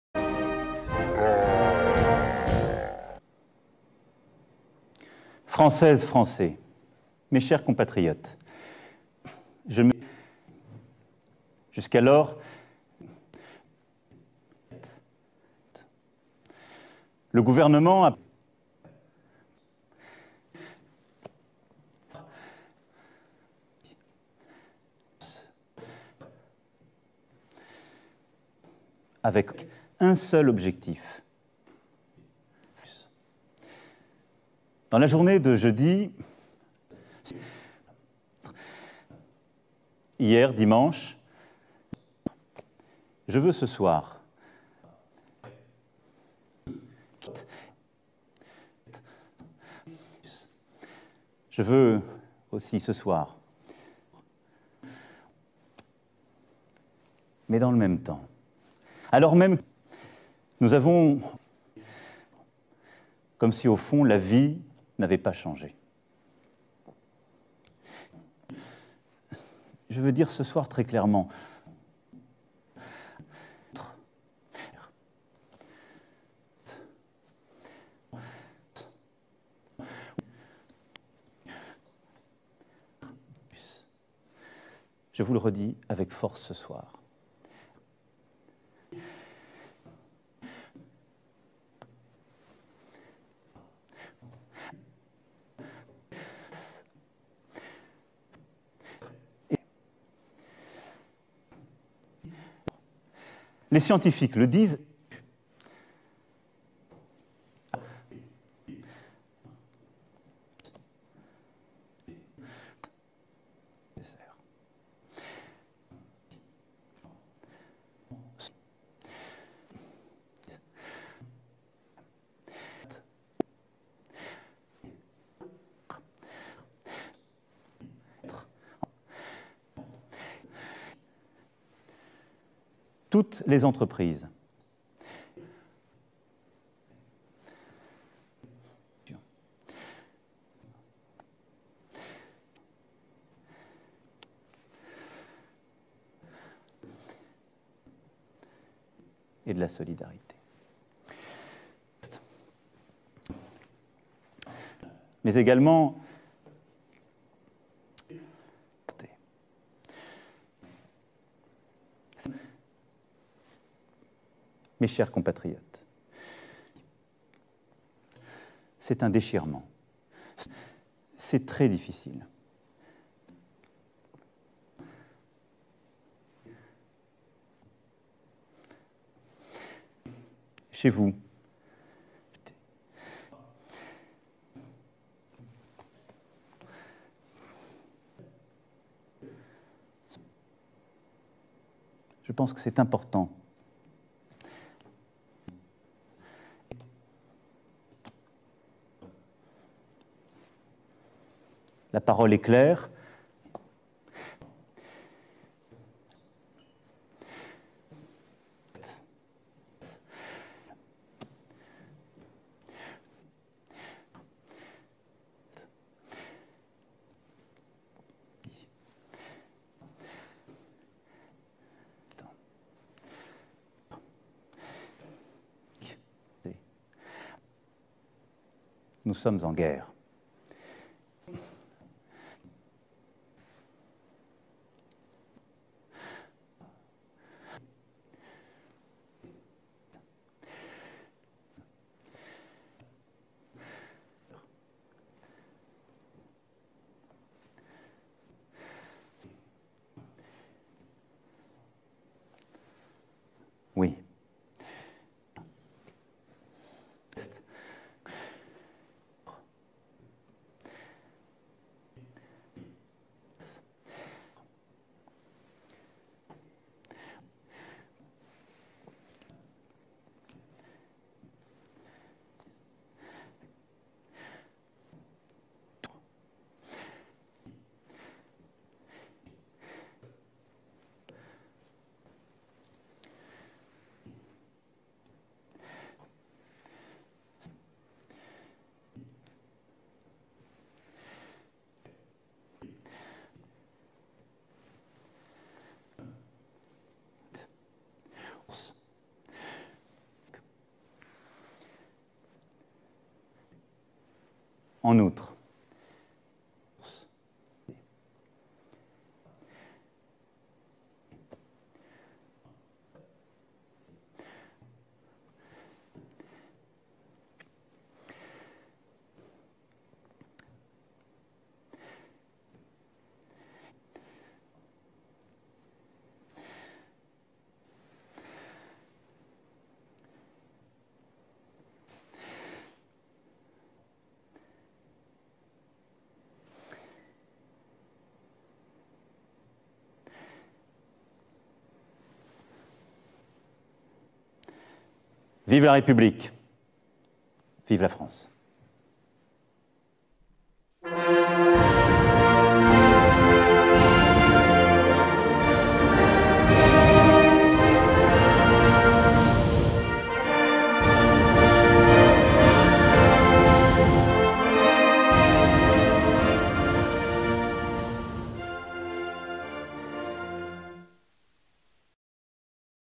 Manu n'a plus d'inspi
Mes chers compatriotes, notre président a voulu s'exprimer devant le peuple français. Mais il n'a pas réussi.

Logiciel de montage : Shotcut (GRATUIT ET OPEN SOURCE)
Caméra : celle de l'Elysée oklm
Micro : celui de l'Elysée oklm
Capture d'écran : j'ai téléchargé la vidéo oklm